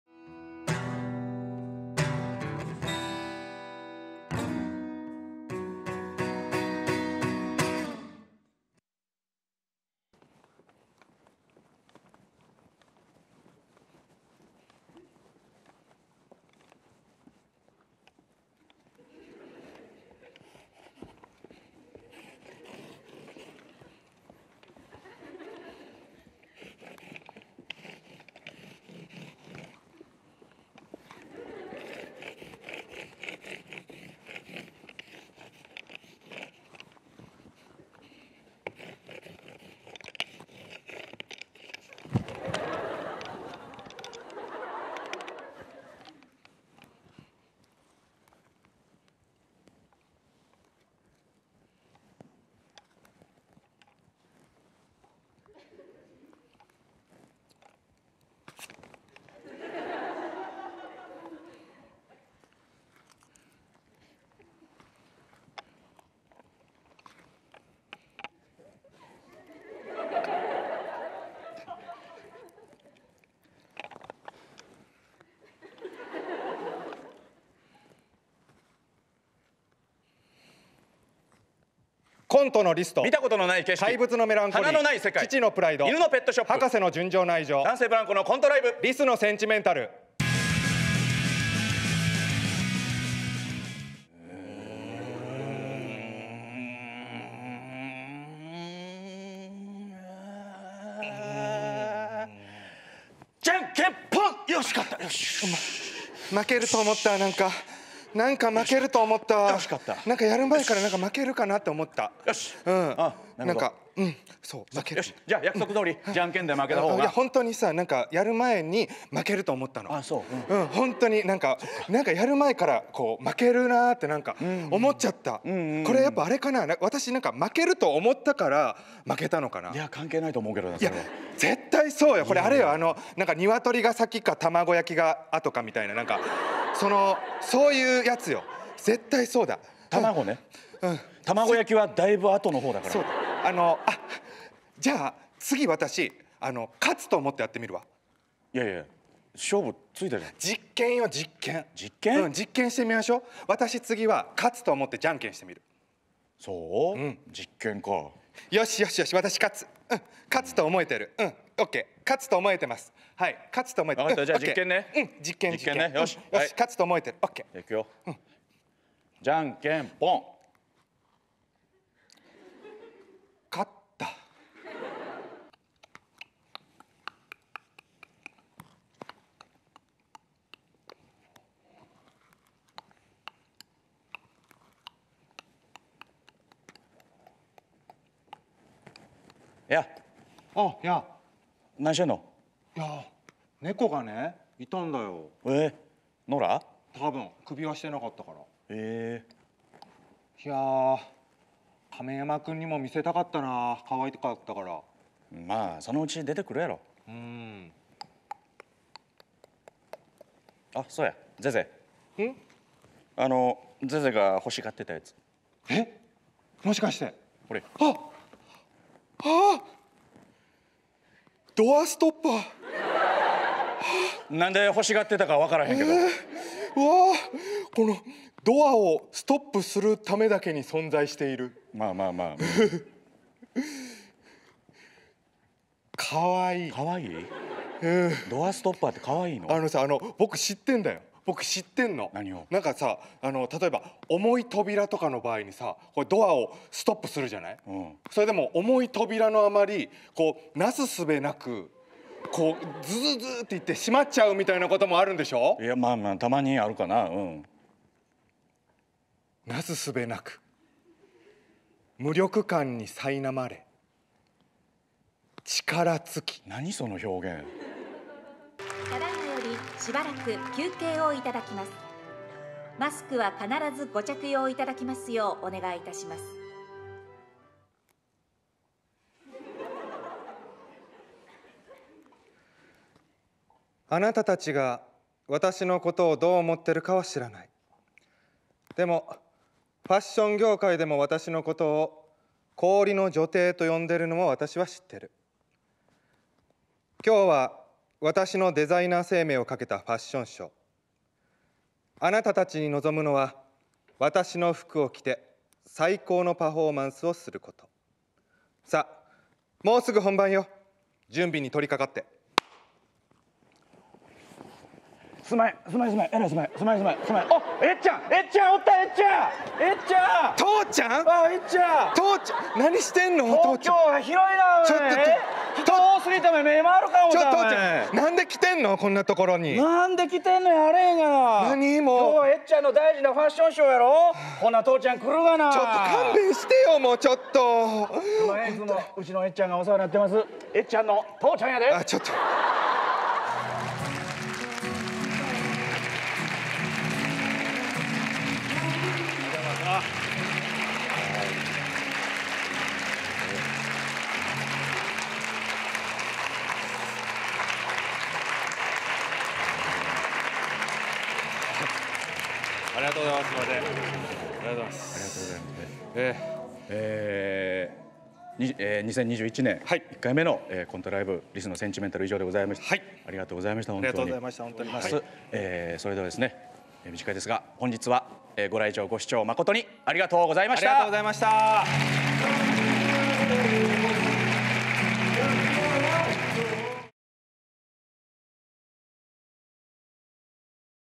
Dansei Blanco’s Conte Live [Squirrel Sentimental]
Up-and-coming conte comedians Dance Blanco present their lastest solo show!
With offbeat moments and oddball characters, lose yourself in the woldbuilding of these two young talents!  With online streaming and in-person viewing, enjoy this show in person or from the comfort of your own home!

実力派若手コント師の「男性ブランコ」による単独ライブを開催！
彼らが作り出すコントは何とも切ないのに笑えたり、癖になるキャラクターも盛りだくさん、どこか懐かしさを感じたりと公演そのものが作品となる独特な世界観です。会場での観劇だけではなく、オンラインでの生配信も行うため全国各地どこでも楽しんでいただけます。